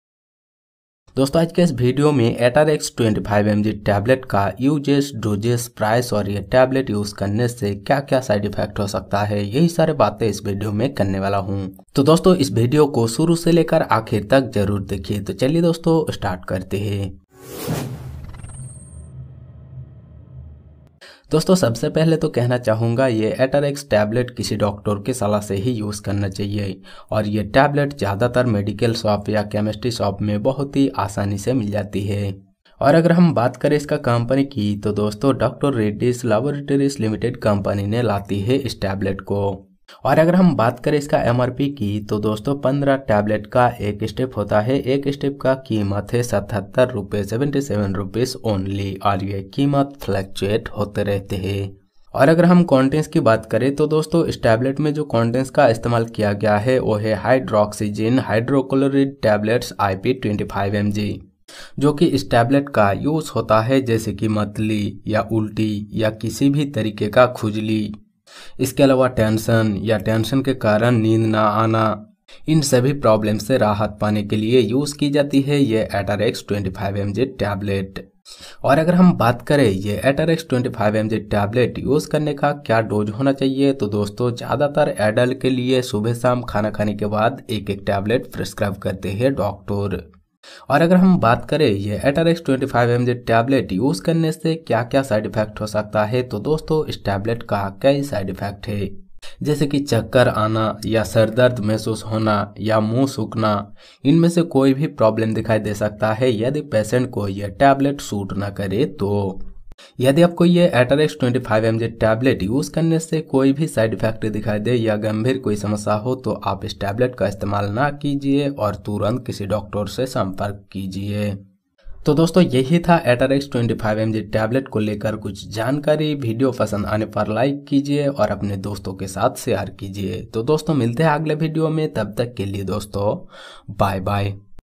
Atarax Tablet Review in hindi | Hydroxyzine Tablet Review in hindi | by Mt discuss
Tablet Name - Atarax
Composition - Hydroxyzine Hydrochloride Tablets
Marketed by - Dr Reddys